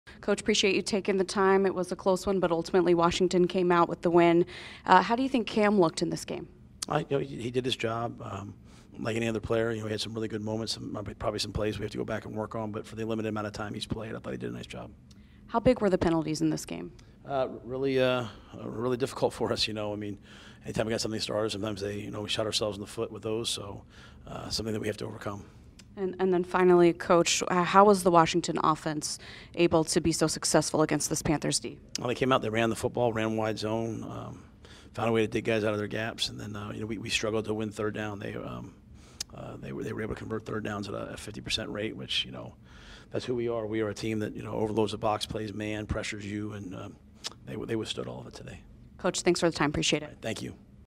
Matt Rhule gives immediate thoughts after the loss against the Washington Football Team
Hear what Matt Rhule had to say after Sunday's game against Washington.